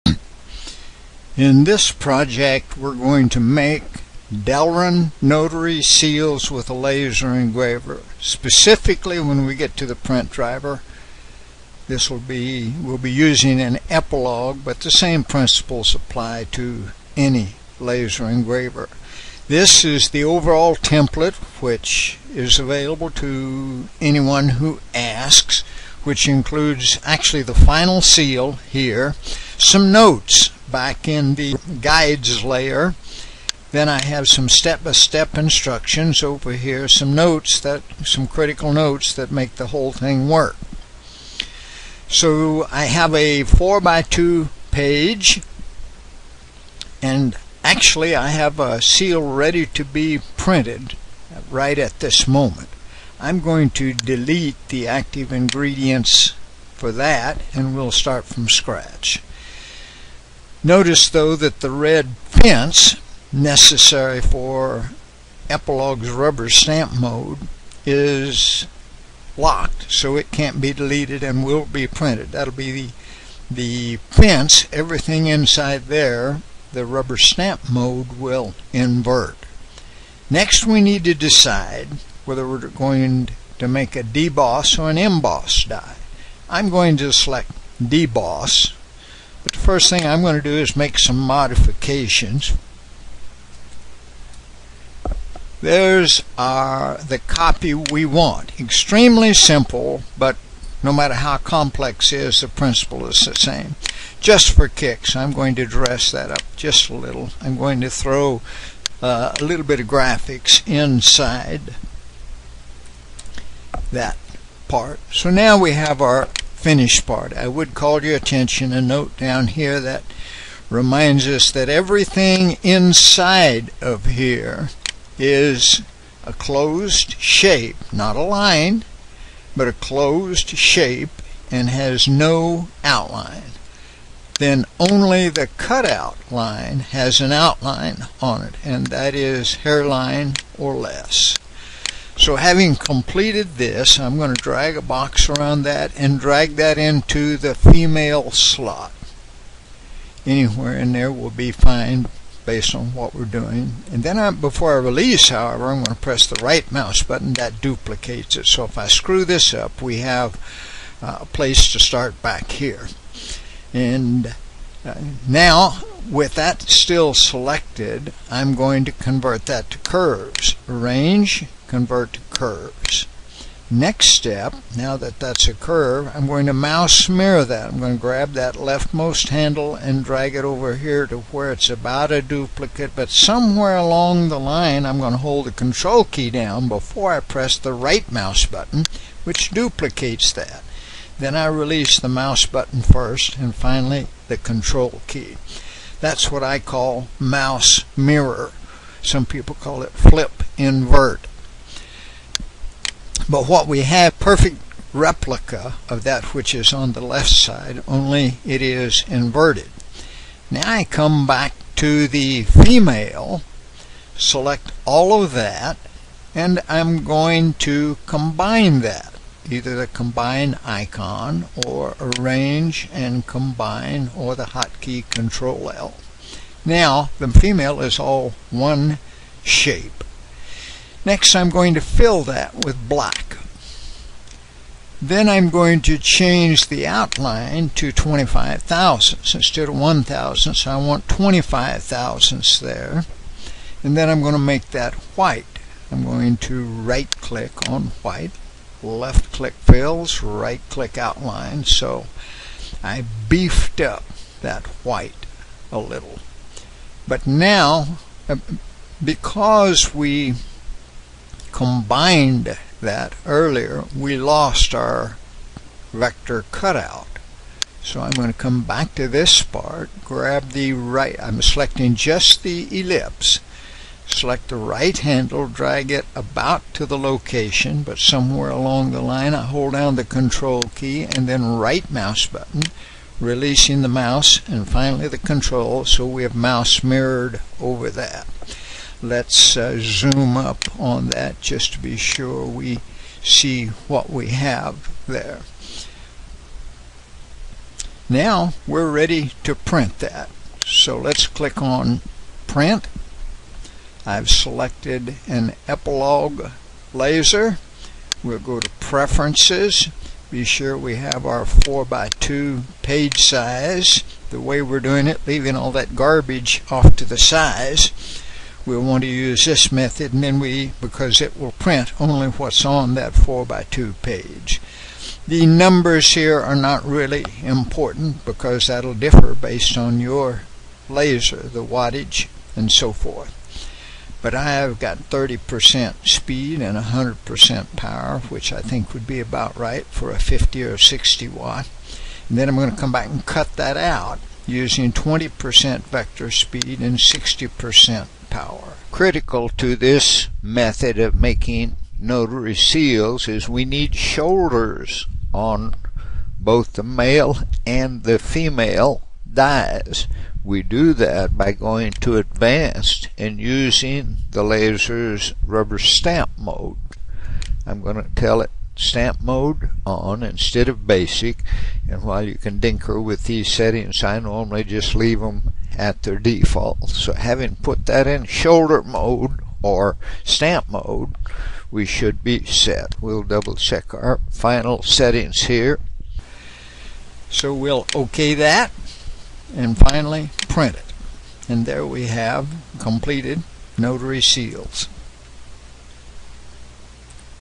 Lasering Delrin Seals
CorelDRAW printing to a laser engraving system to create embossiing dies(male-female pair) for -Notary- type seals.